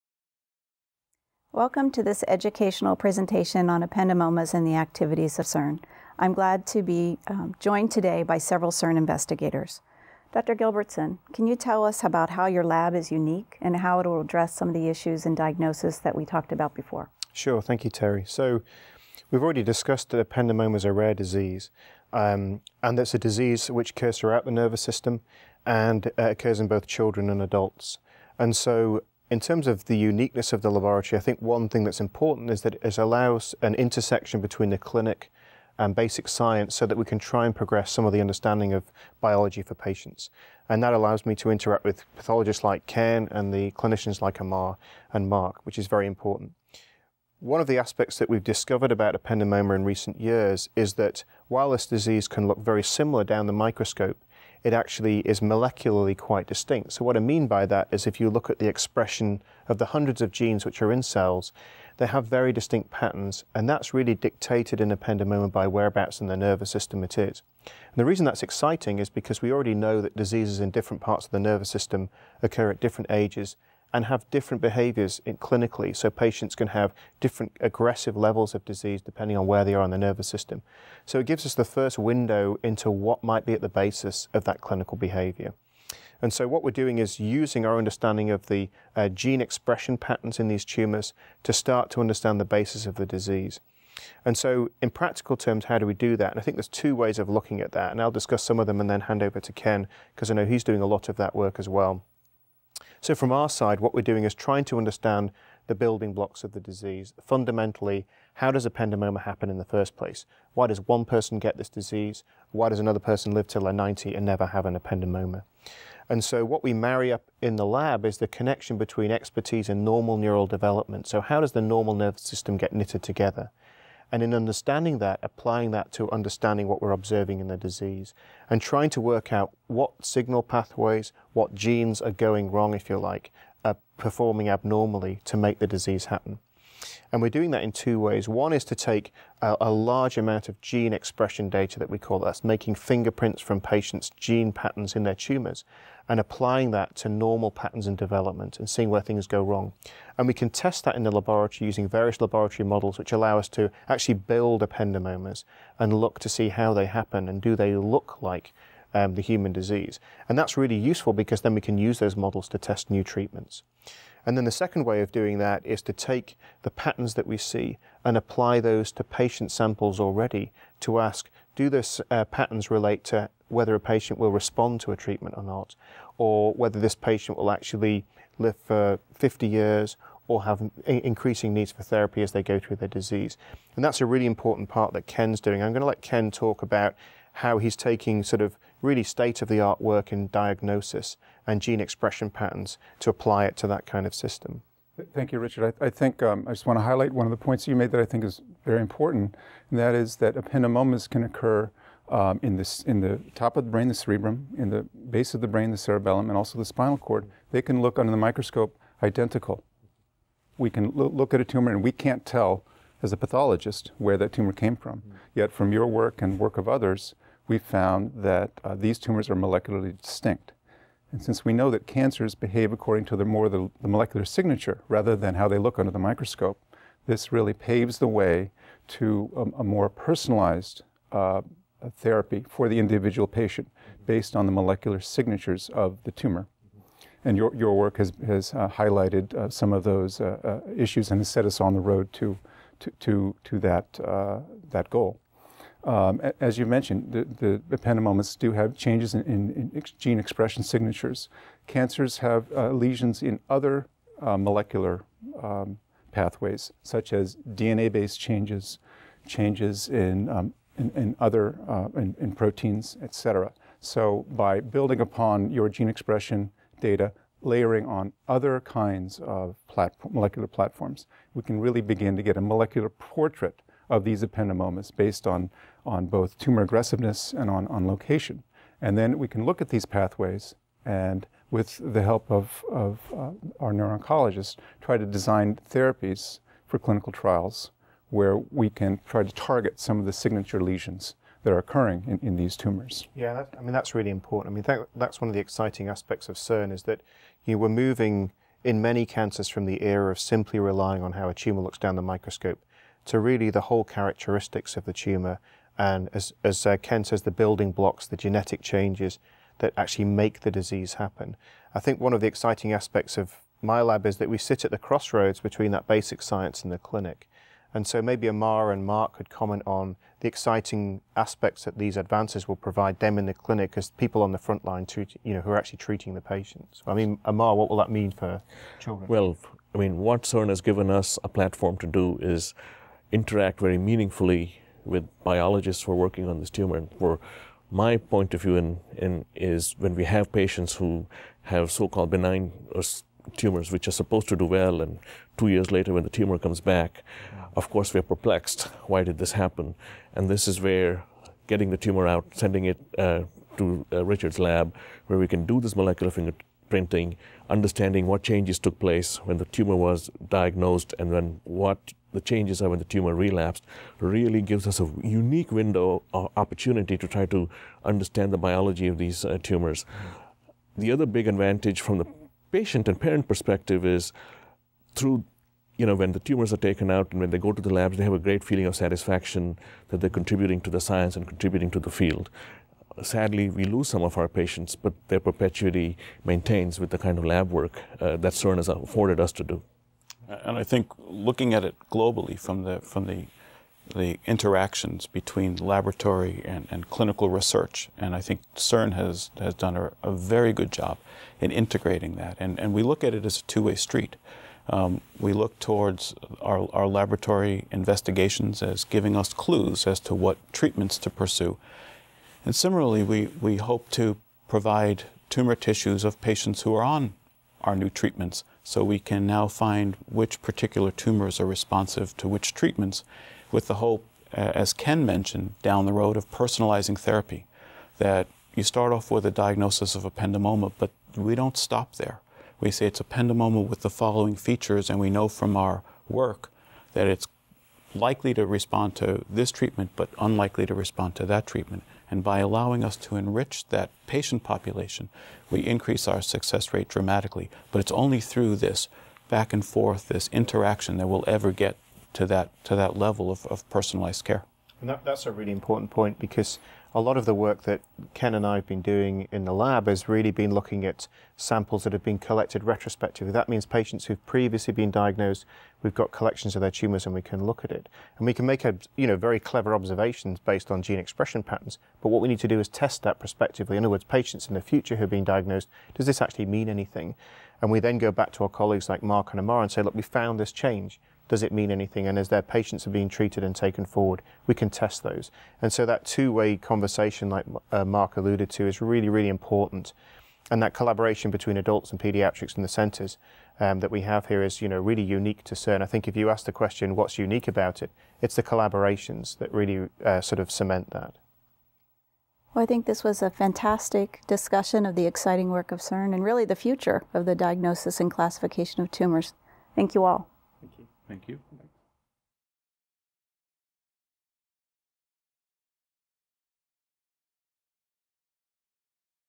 Research on Ependymoma from The CERN Foundation
Dr. Terri Armstrong sits down with fellow CERN Members including Dr. Amar Gajjar, Dr. Richard Gilbertson, Dr. Ken Aldape, and Dr. Mark Gilbert to discuss the exciting science currently being done in ependymoma research. The CERN Foundation is unique in its focus on ependymoma but also in its willingness to collaborate across institutional lines.  Learn more about ependymoma and the CERN Foundation You can join our e-newsletter list to receive updates on ependymoma and join the message boards.